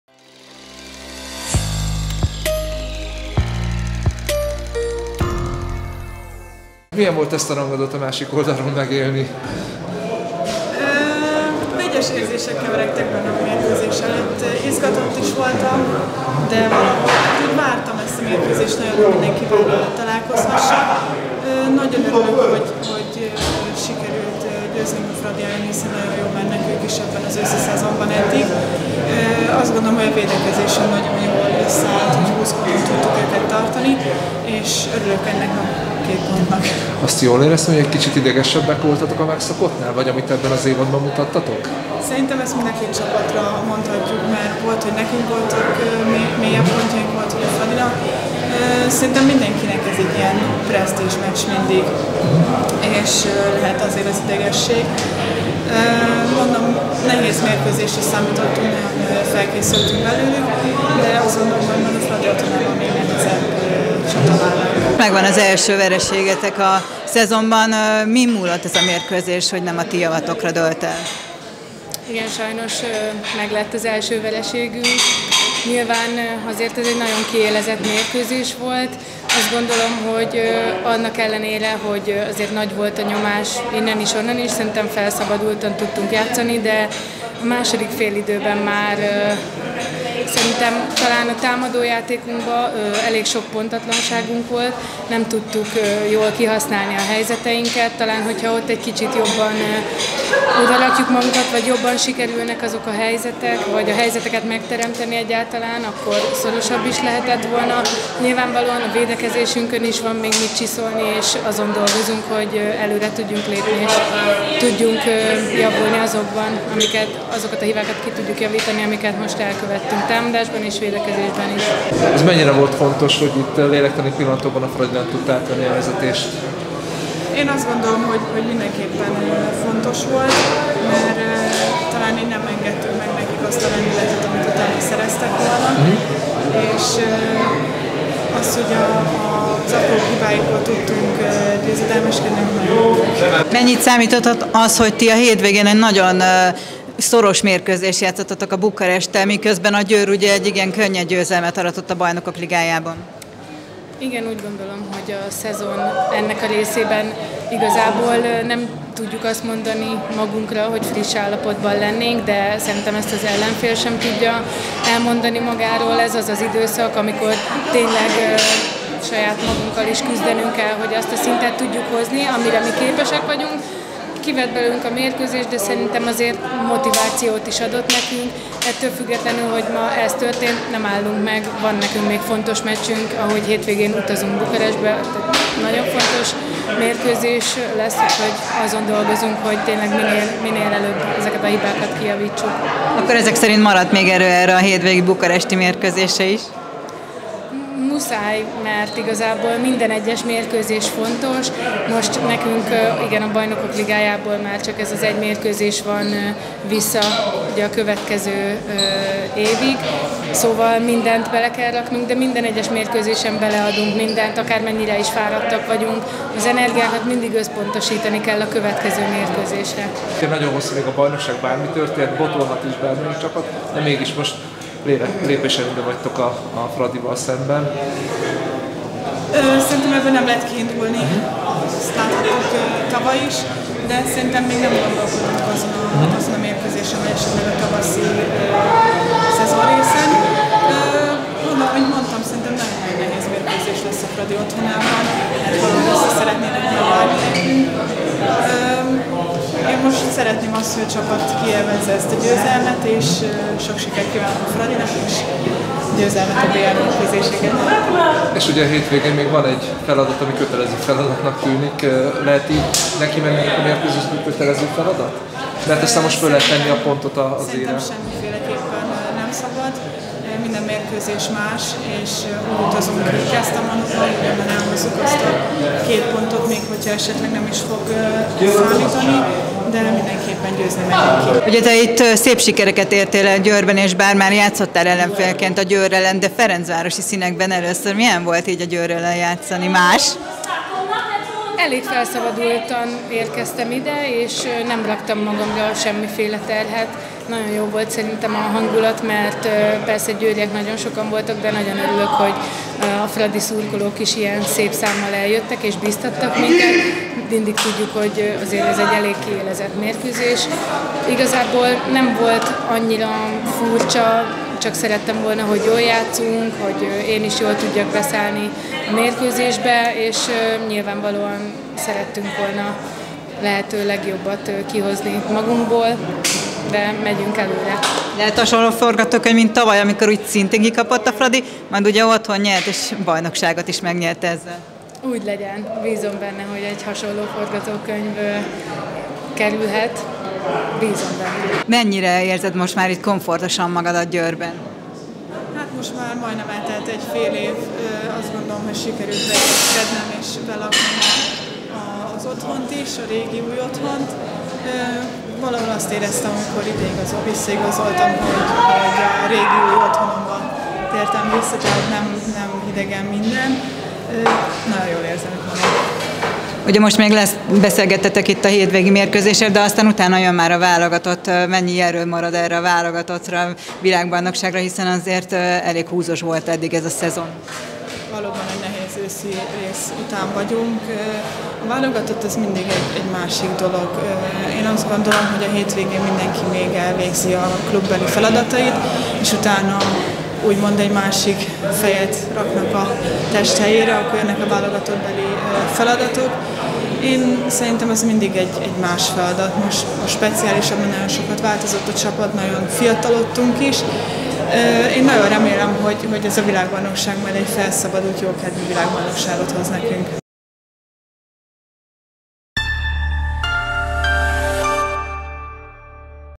Schatzl Nadine: Vegyes érzések kavarogtak bennem a Fradi ellen
A Győr 25–20-ra győzte le az FTC-t a női kézilabda-bajnokságban. A mérkőzés után a Fradiből a Győrbe visszatérő Schatzl Nadine, és a győriekkel korábban BL-t is nyerő Hársfalvi Júlia értékelt az NSO Tv-nek.